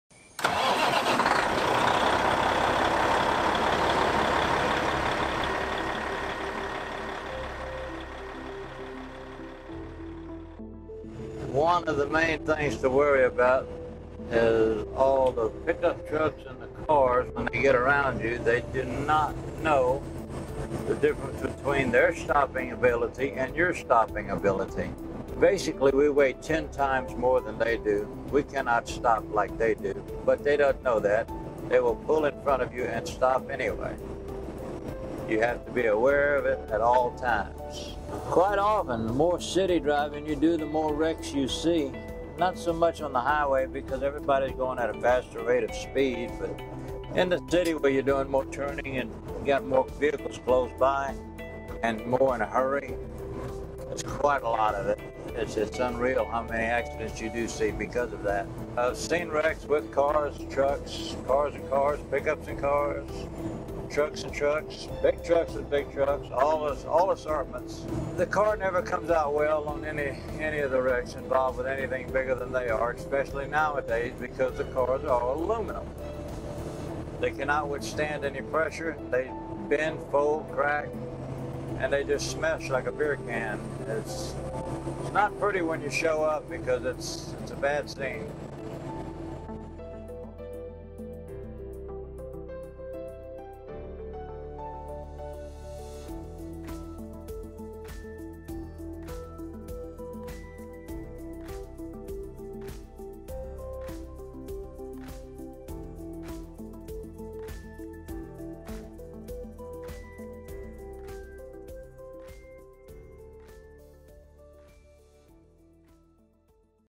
Roadside Awareness PSA - Inside The World of Truckers
Short PSA video for TAMUCC Commercial Production Course. The video gives a small glimpse into the life of a commercial driver, the obstacles they face, and the experiences they have over the road.